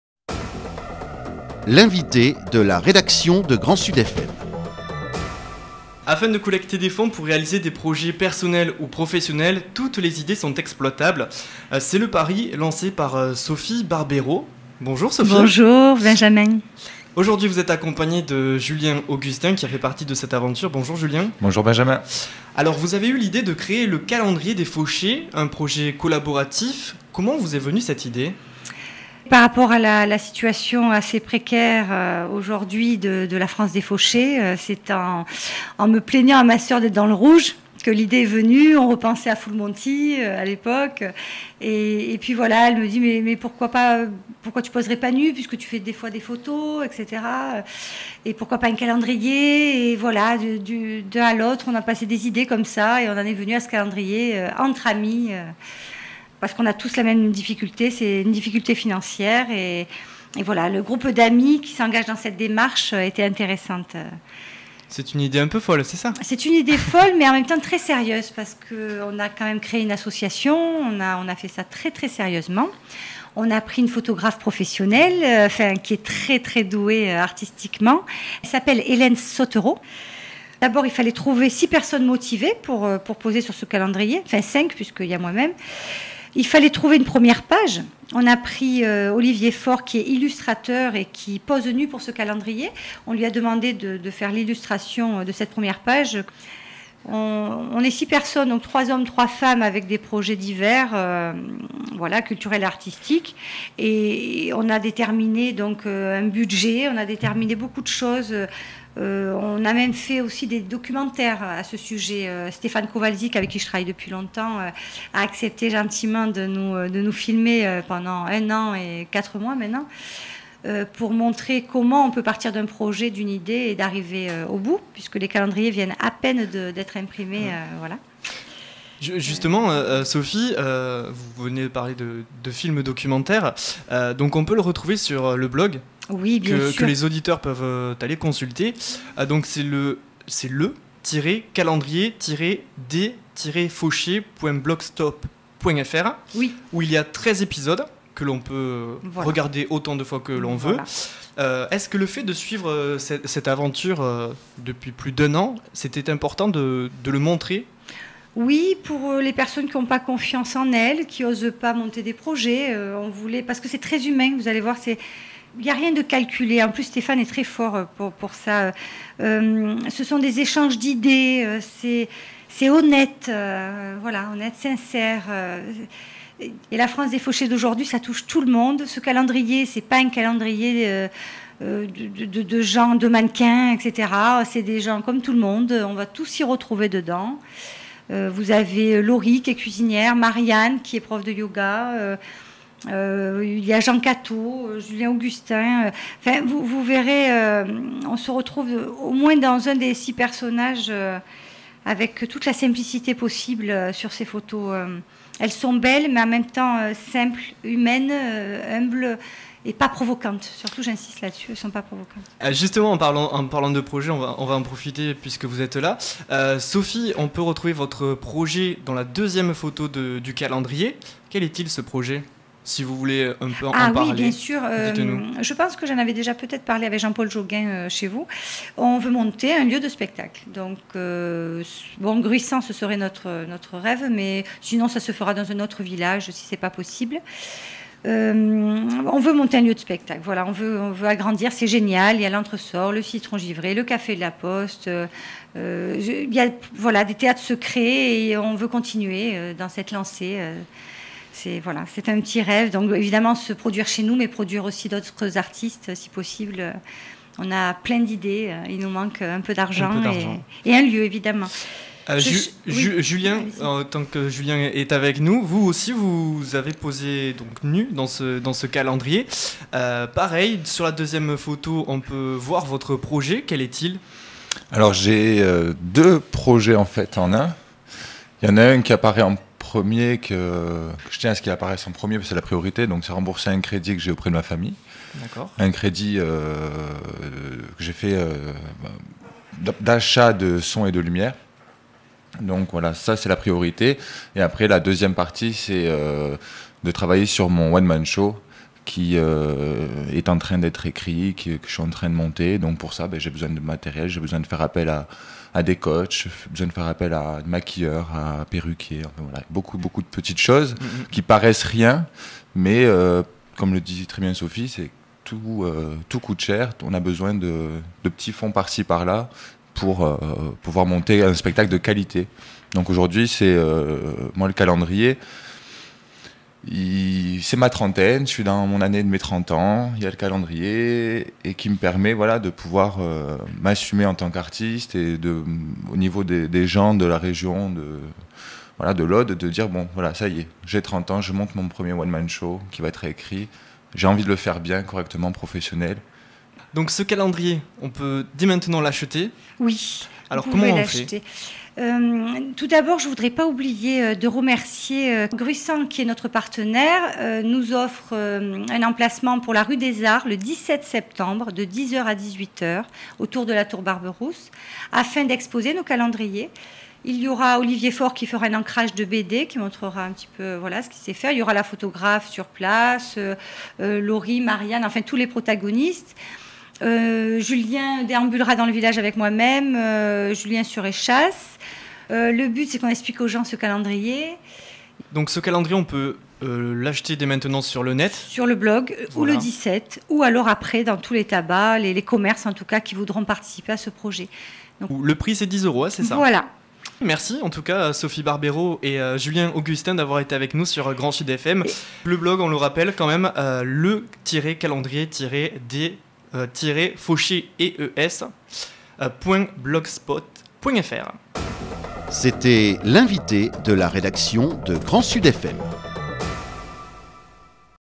Sophie et Julien présentent le calendrier des fauchés sur Grand Sud FM # 14
Le calendrier des fauchées est un projet collaboratif destiné à collecter des fonds dans le but du financement des projets. Aline et Sophie Barbero ont eu l'idée de réaliser un calendrier avec des femmes et des hommes photographiés nus. Elles m'ont demandé de suivre leur projet et de réaliser un documentaire sur cette aventure sociale et artistique.